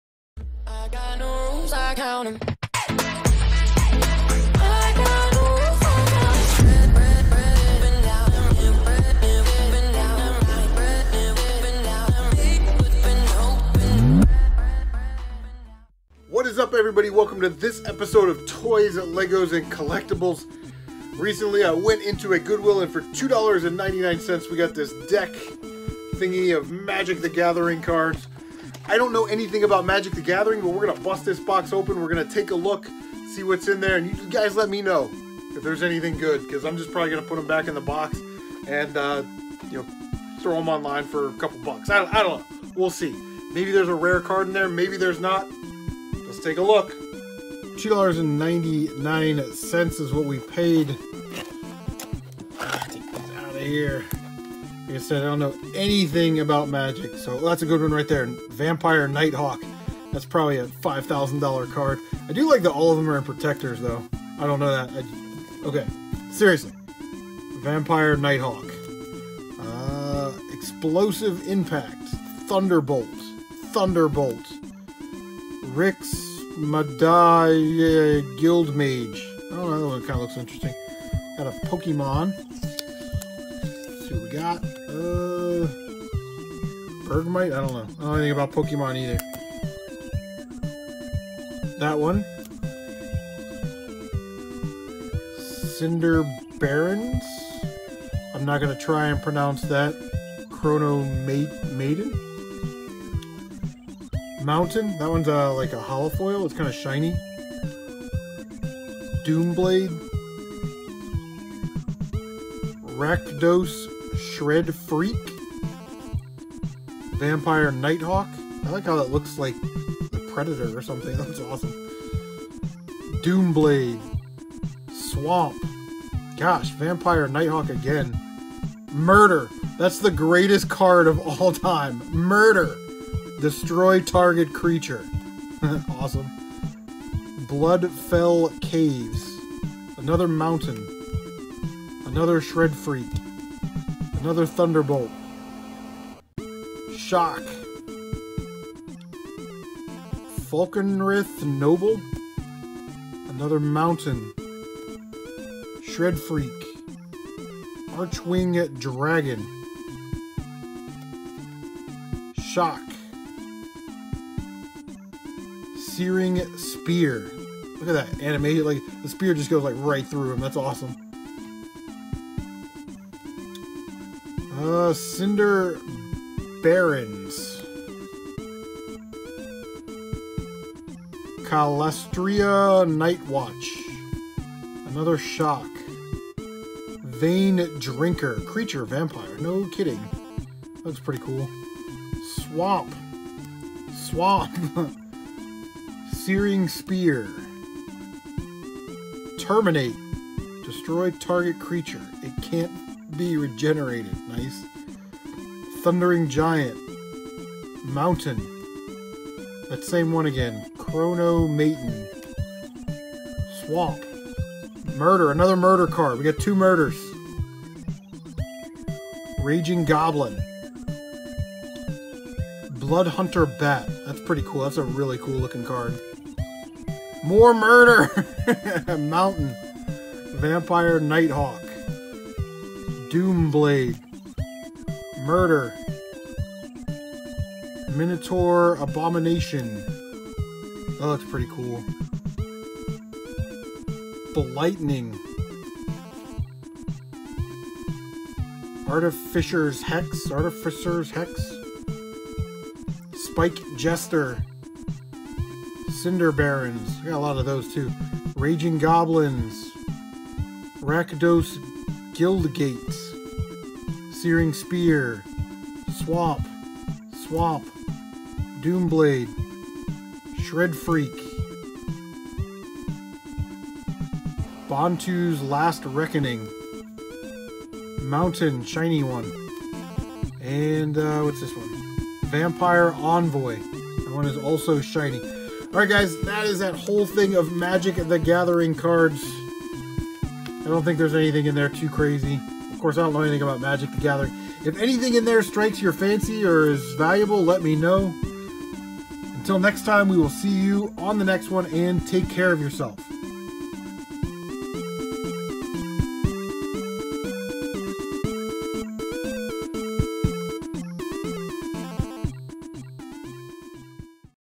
Opening a Random Box of Magic the Gathering Cards - TLC Episode #9
In this episode I'll open a random box of Magic the gathering cards that I got a Goodwill for $2.99. Let's take a look.